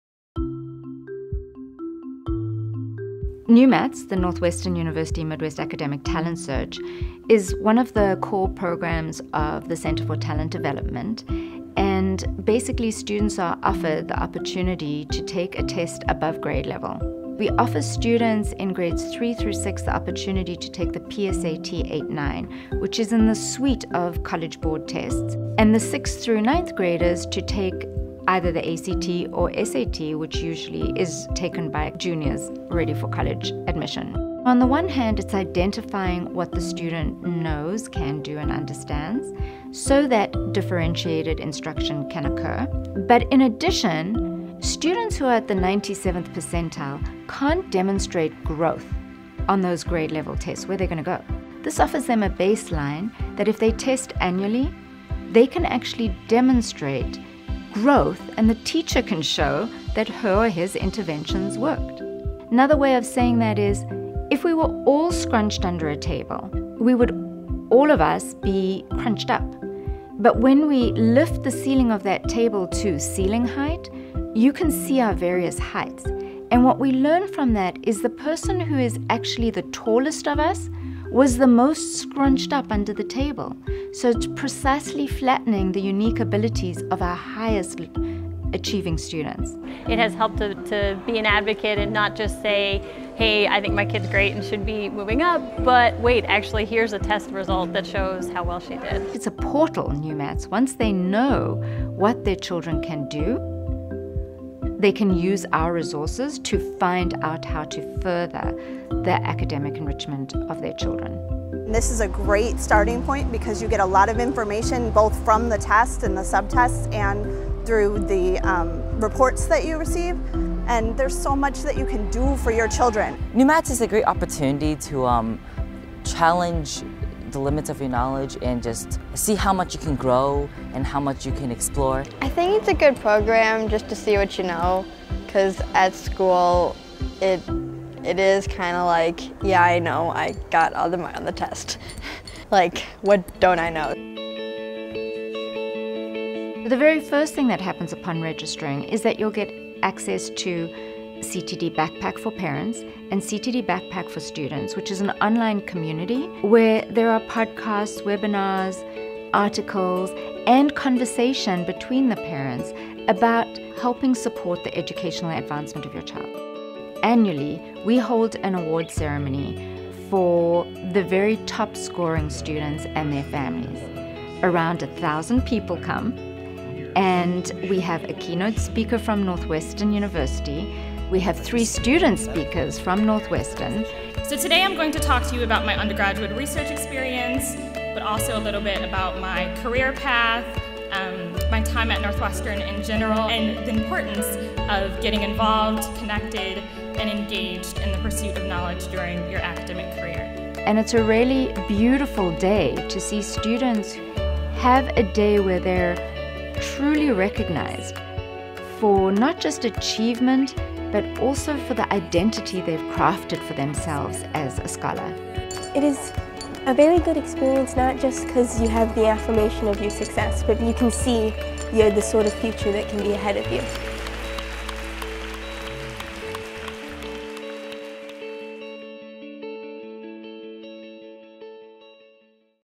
Northwestern University's Midwest Academic Talent Search (NUMATS), Center for Talent Development
Northwestern University's Midwest Academic Talent Search (NUMATS) offers research-based assessment to identify academic ability, measure growth, and connect students to resources and opportunities for advanced and gifted students.